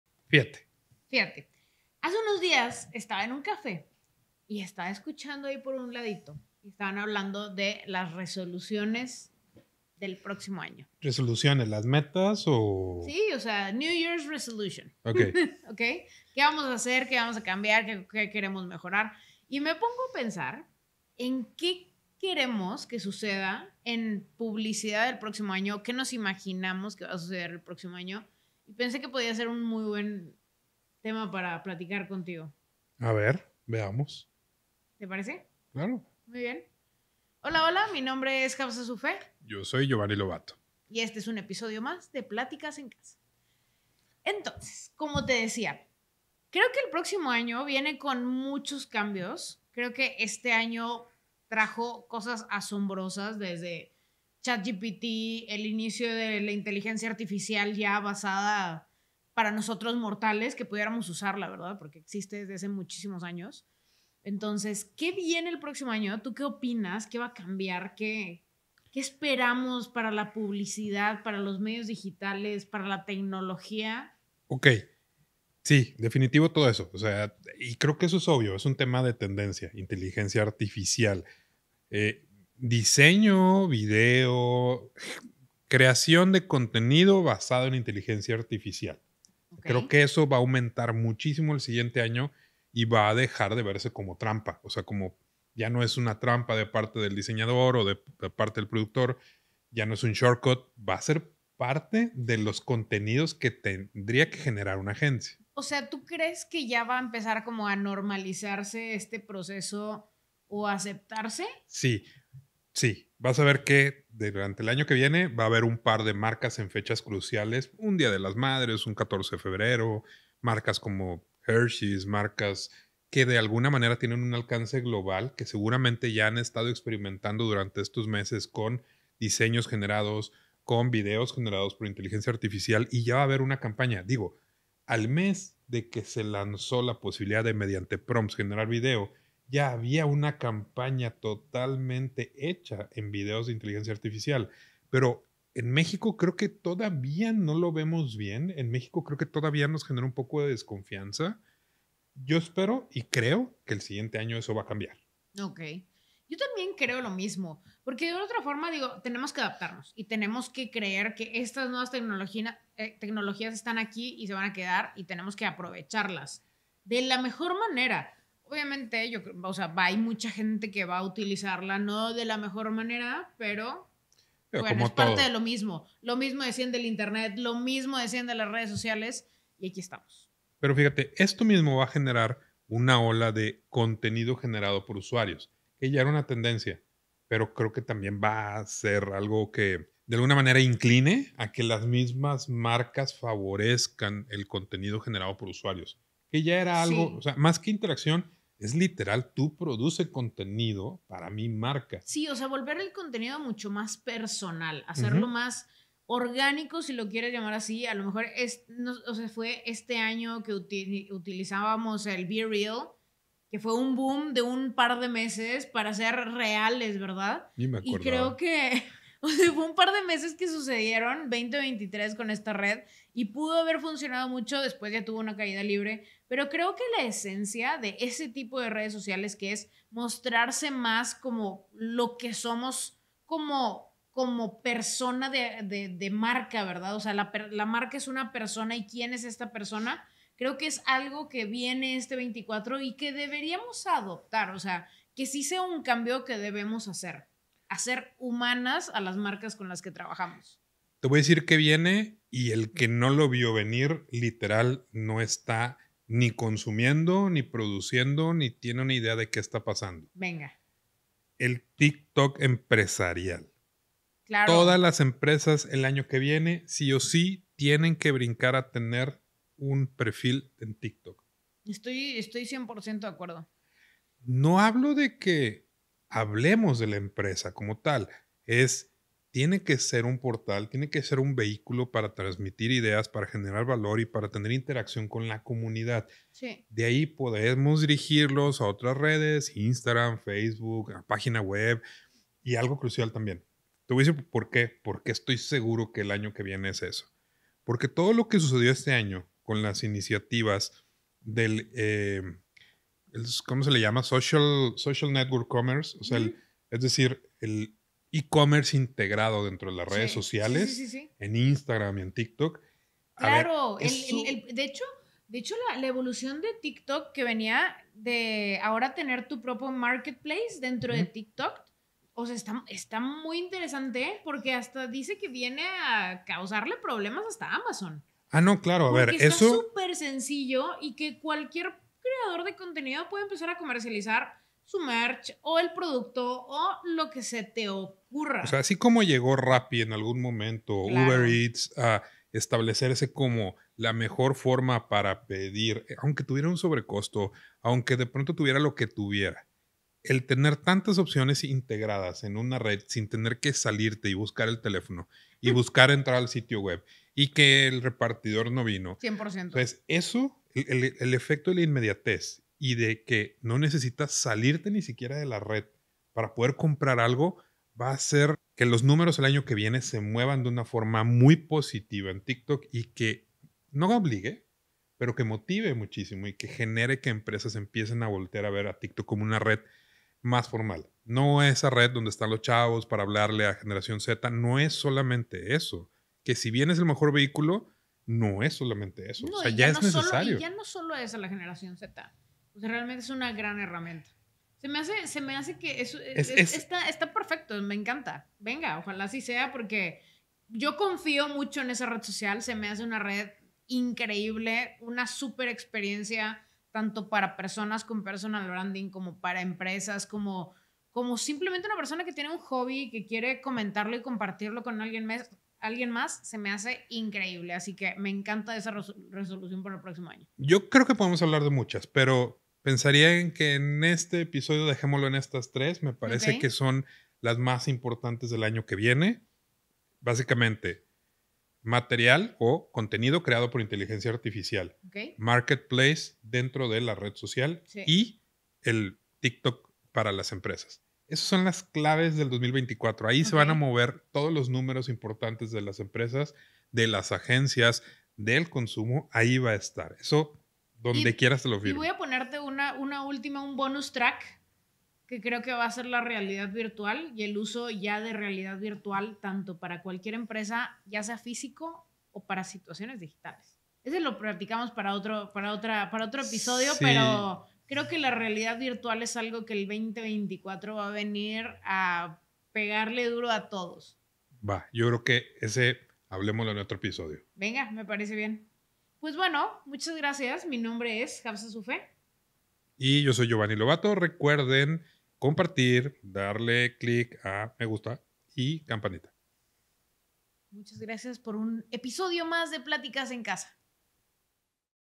Platicas en Casa Ep. 29 - Metas de año nuevo para la publicidad
¡Se acerca el nuevo año! Hablemos de lo que esperamos y queremos para el año próximo en cuestión publicitaria.